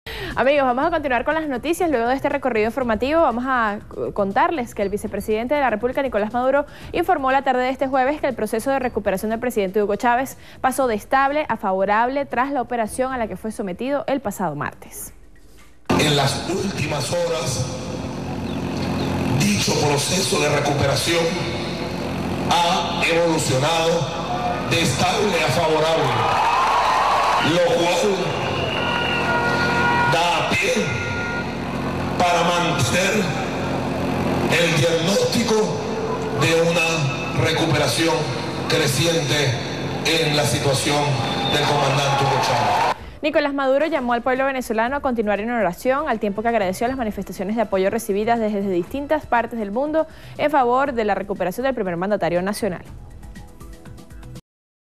Maduro sostiene que proceso de recuperación de Chávez pasó de "estable a favorable"
El vicepresidente de la República, Nicolás Maduro, informó que el presidente de la República, Hugo Chávez, se encuentra en proceso de recuperación y su estado, según el último parte médico, pasó de "estable a favorable".